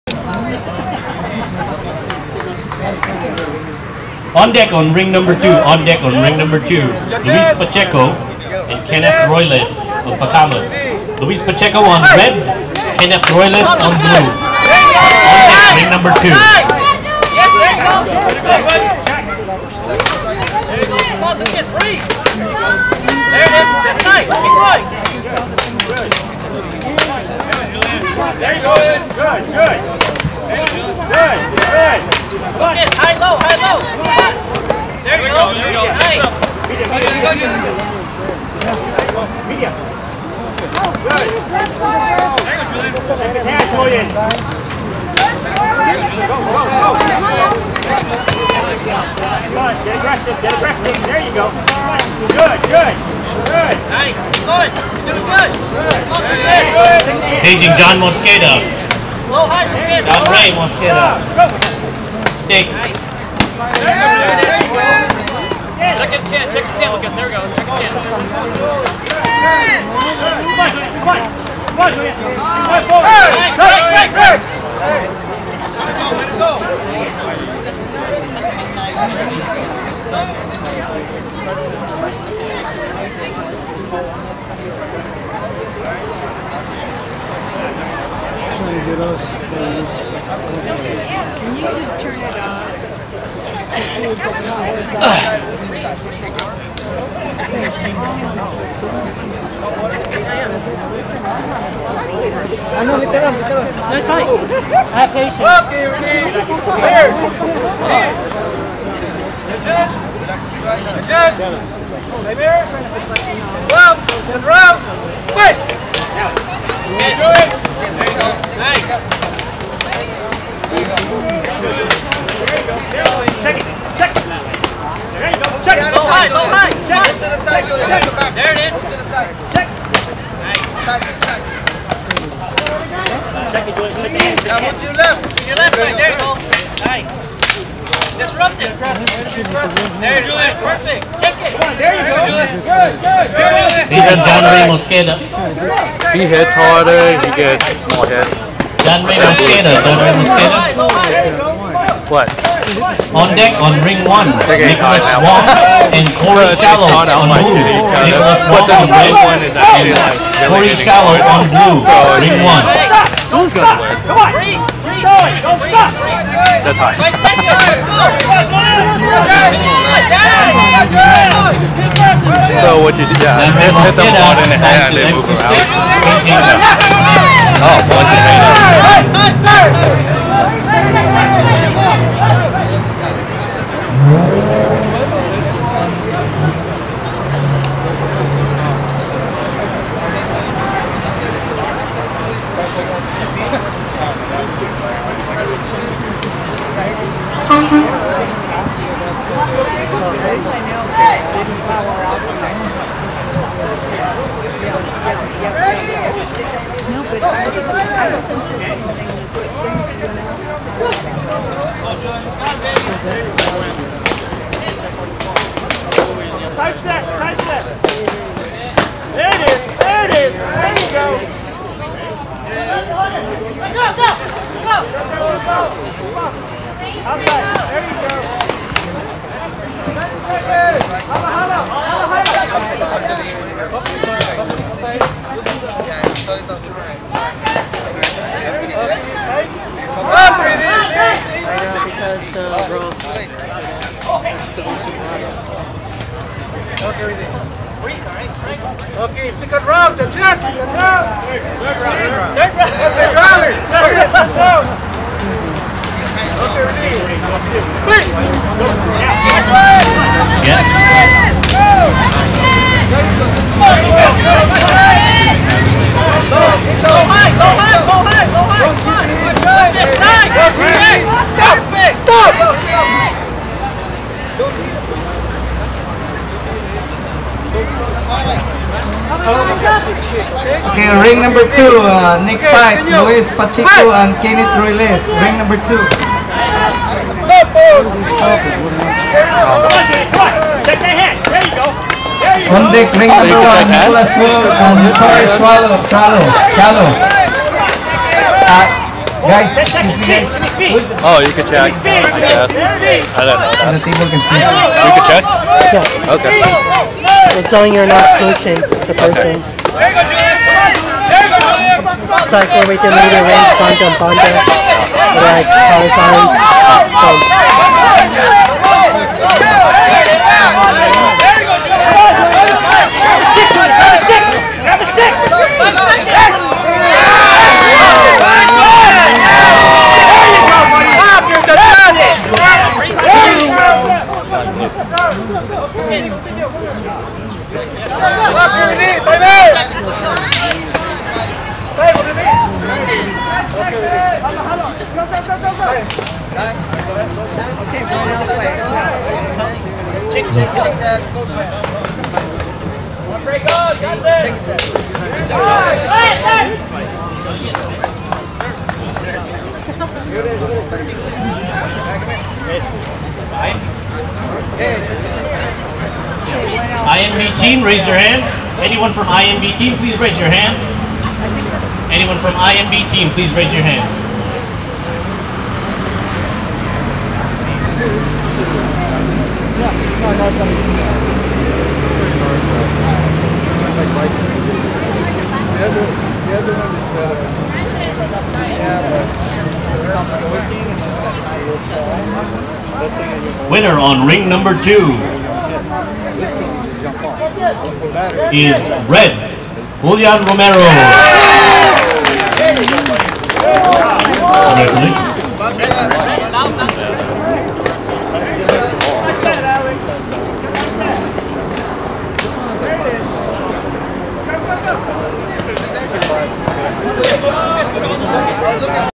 Single live stick sparring at the Los Angeles 1st annual FMA tournament 2/7
10/14/2017
Filipino American heritage month festival

Single live stick sparring part 2

/The tournament is hosted by Master Erwin Mosqueda of the LA Doce Pares system.